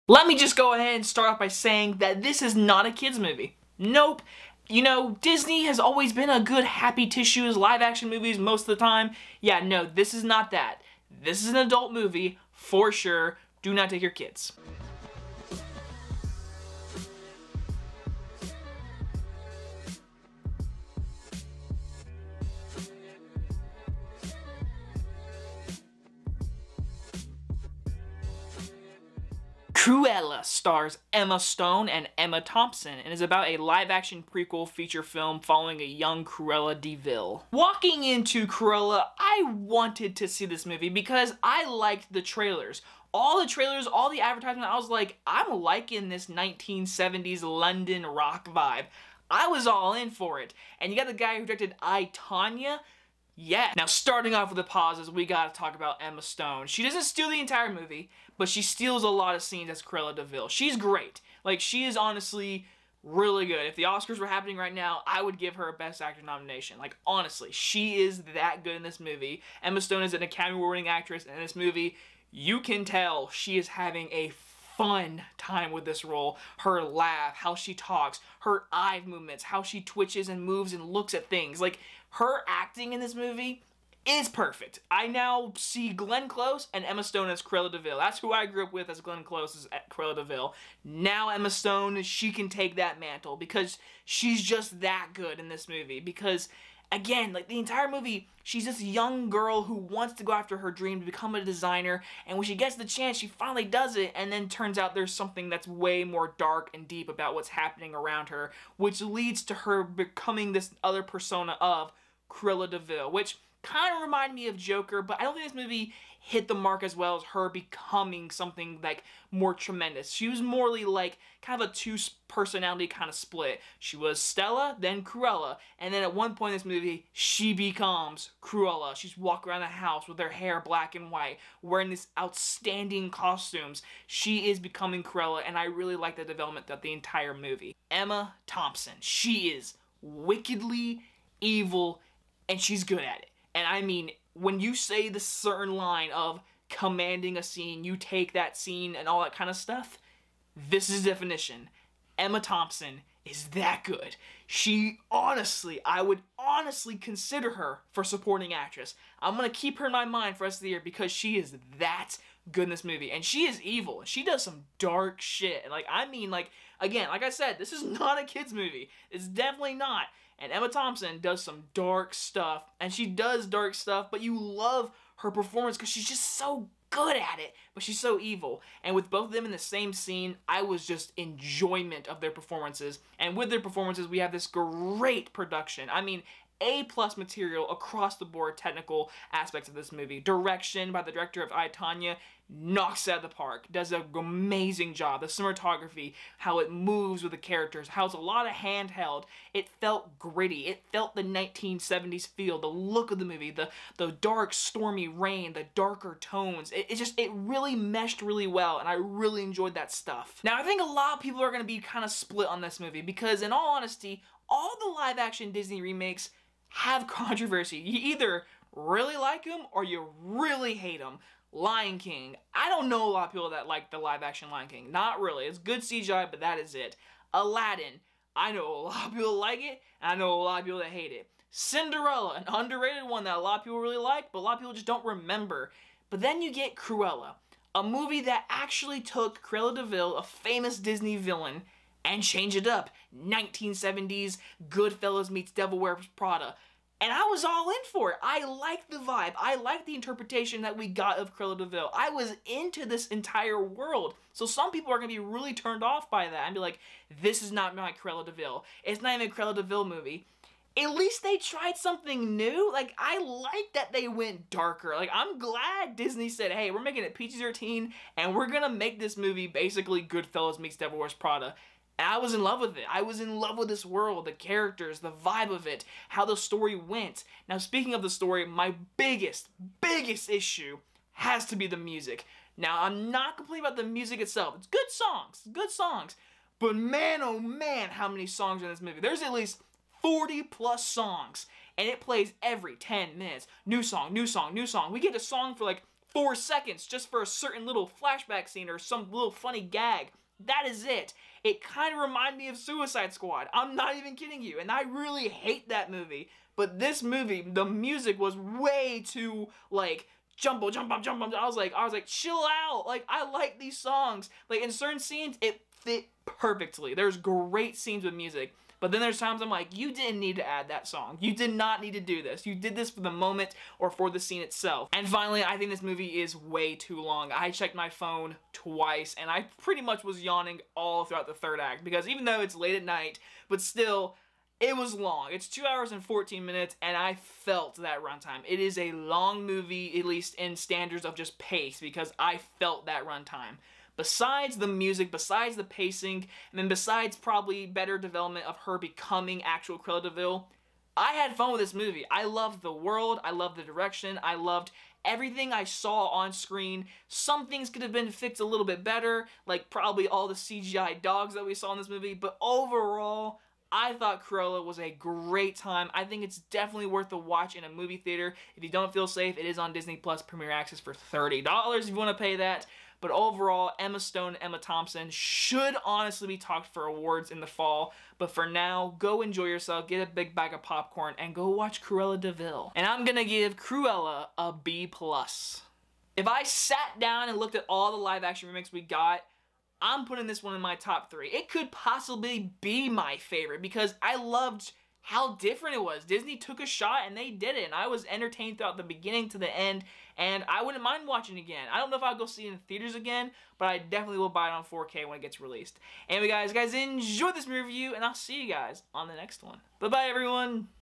Cruella - Movie Review
Emma Stone stars in this brand new live action remake of Disney catalog continue the trend of being bad, now this time we have something very new and creative.